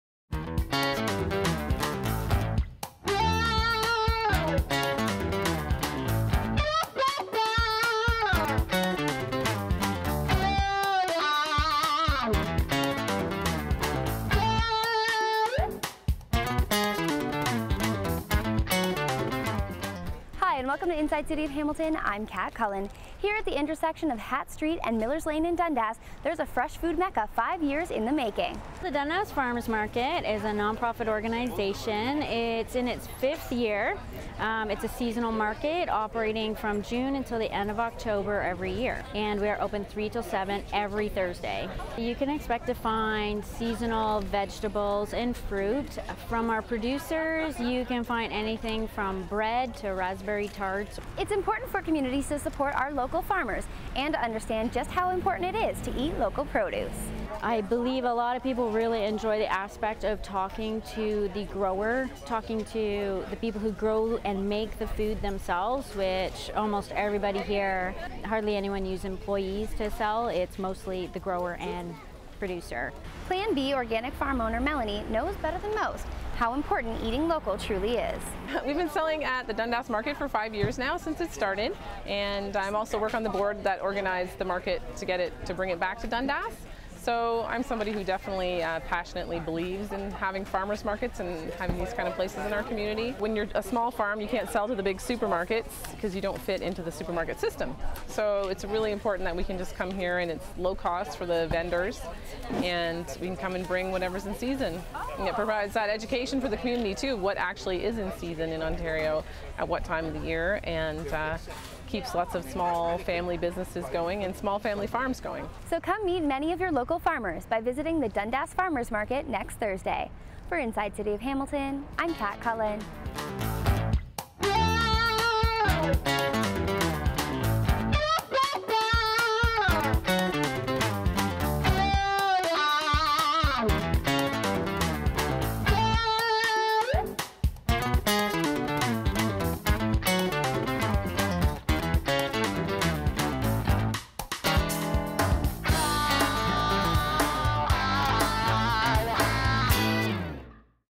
Dundas Farmers Market
Downtown Dundas is bustling with the sights and sounds of the local farmers market. Come visit this thriving market to pick up your fresh local produce and meet the farmers in your area!